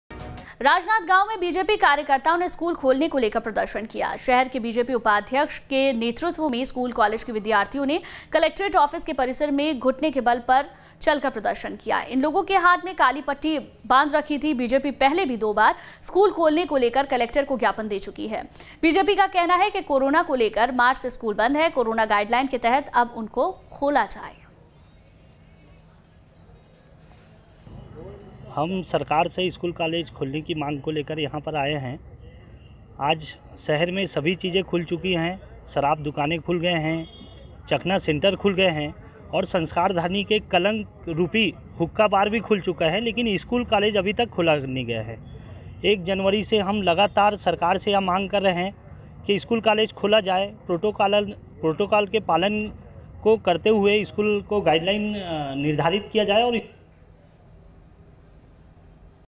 Chhattisgarh: राजनांदगांव में स्कूल खोलने को लेकर बीजेपी की प्रदर्शन, देखें रिपोर्ट
News State Apps डाउनलोड करें:
Download Android and iOS Apps for more updates: